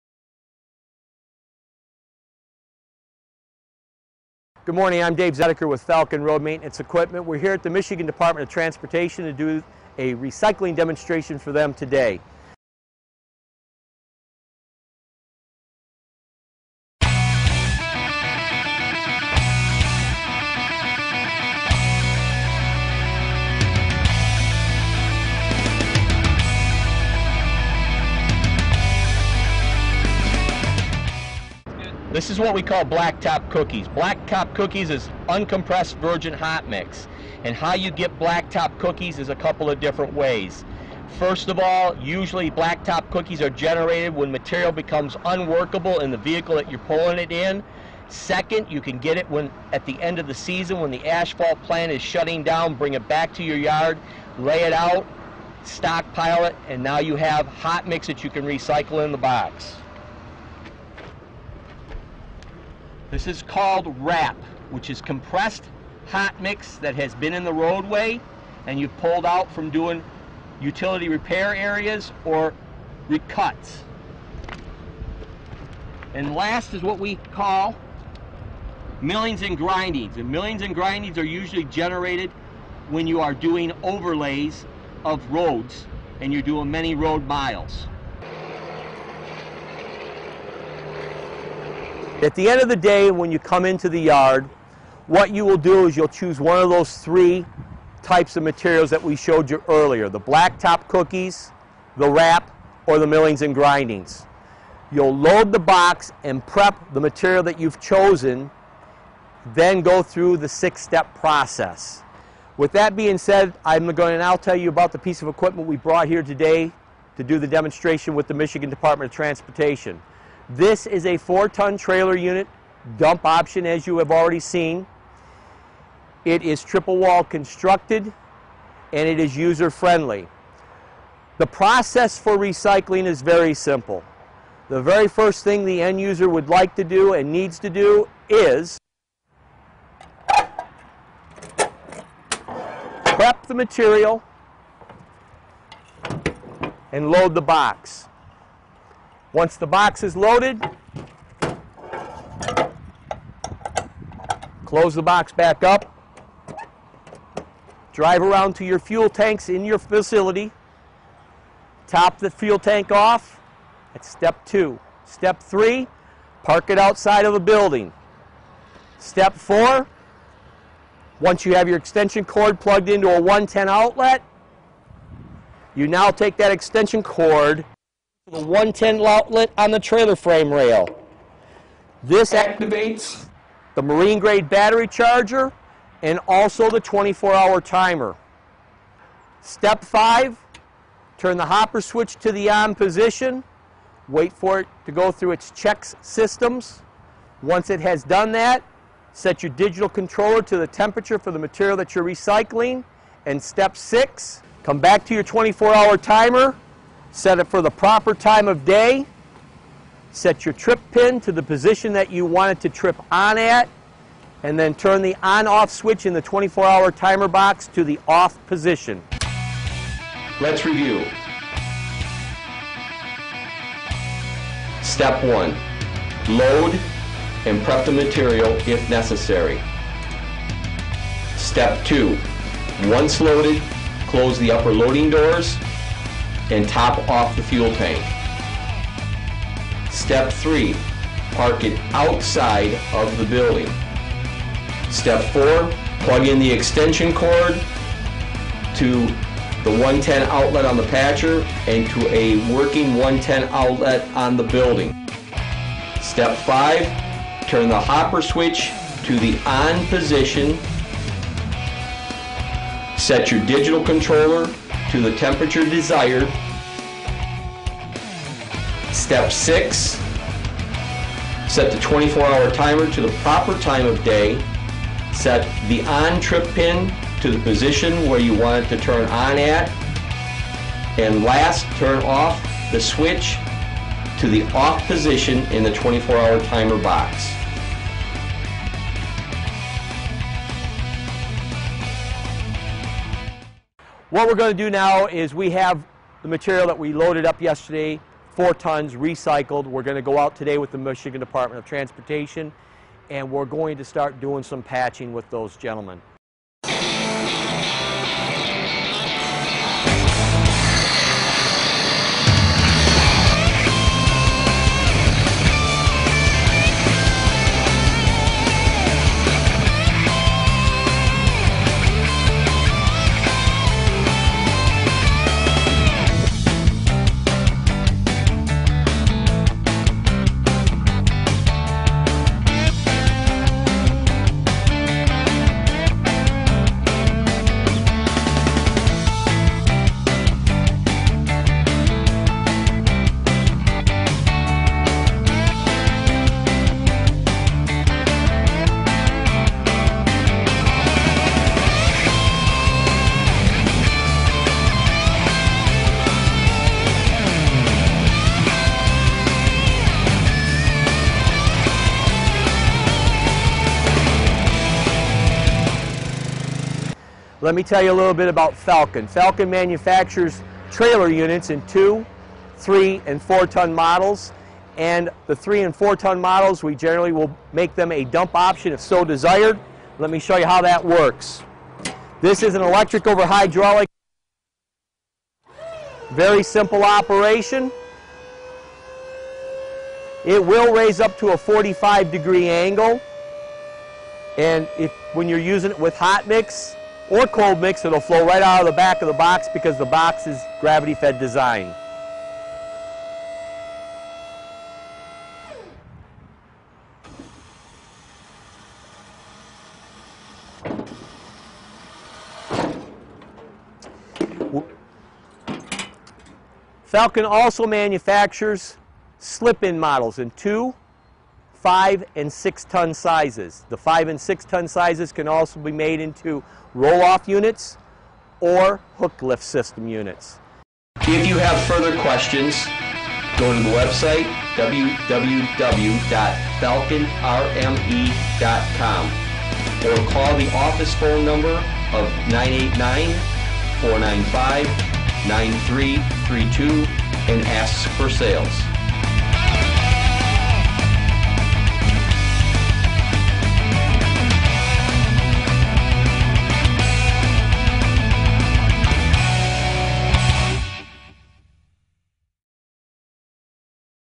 Recicladoras de Asfalto_VIDEO_English.mp4
Video mostrando las Recicladoras de Asfalto marca FALCON.  Este video fue grabado durante una demostración hecha para el Departamento de Transporte del estado de Michigan, USA.  En este pequeño segmento se puede observar la versatilidad y el fácil funcionamiento de esta maquinaria.